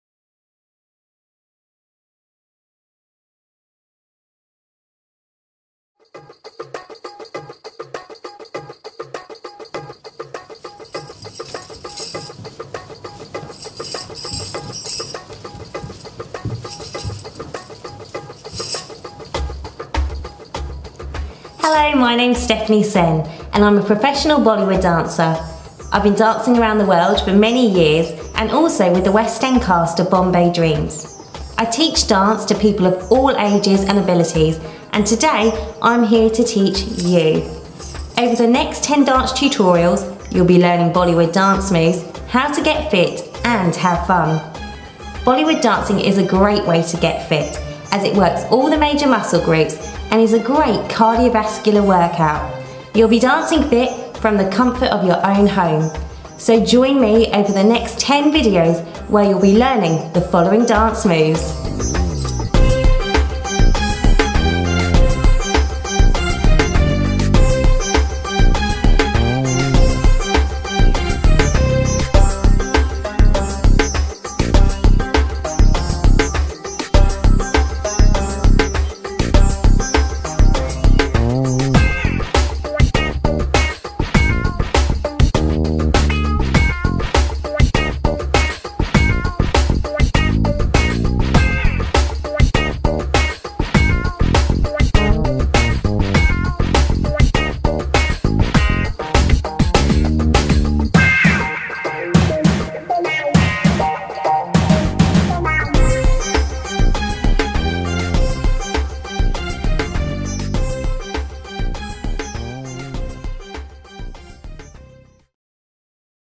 Stephanie Sen - Bollywood Dance Fitness Trailer
Stephanie Sen is an actress and dancer and has been teaching Bollywood dancing for many years. This video is a trailer for a series Bollywood Dance & Fitness sessions for you enjoy, get fit and learn to dance Bollywood style.